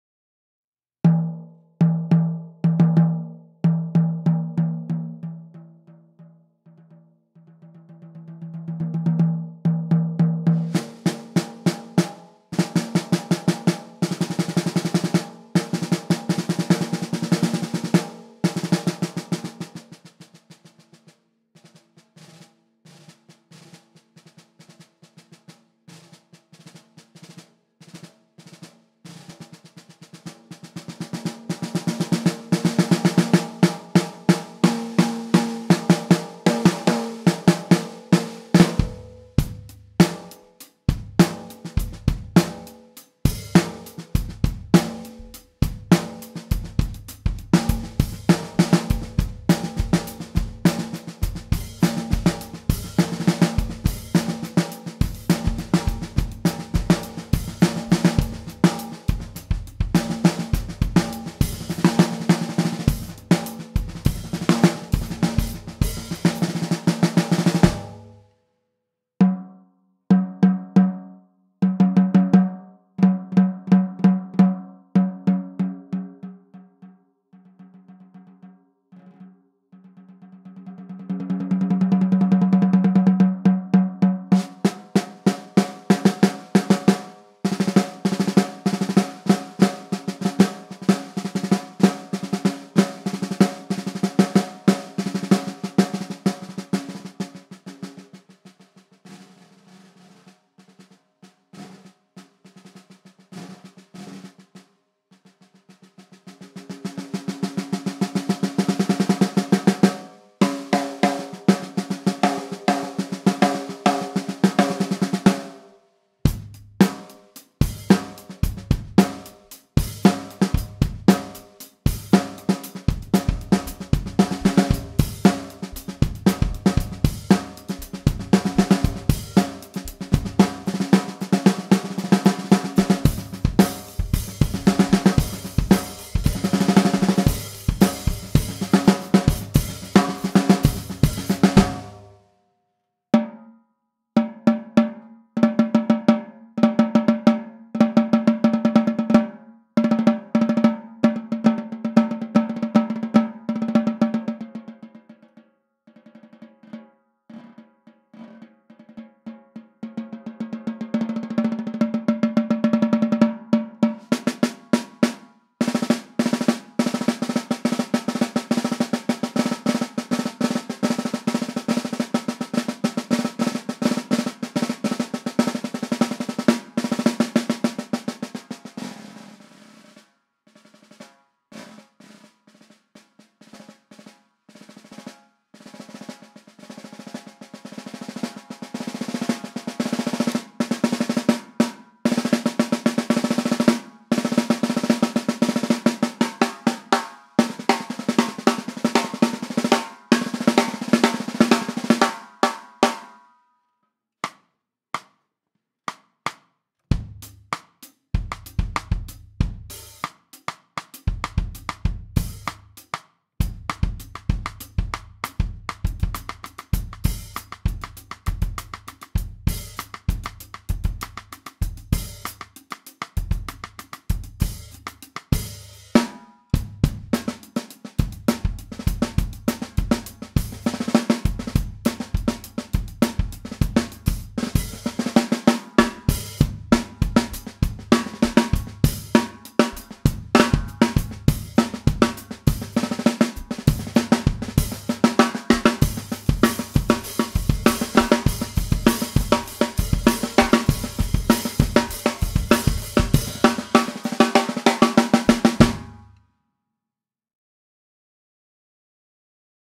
Rogers 14x5 Dyna-Sonic Snare Drum with Beavertail Lugs - White Marine Pearl (36WMP)
Rogers 14x5 Dyna-Sonic Snare Drum with Beavertail Lugs - White Marine Pearl at Memphis Drum Shop

Other gear used in this video:
• Cymbals: Zildjian 14" K Sweet Hi-Hats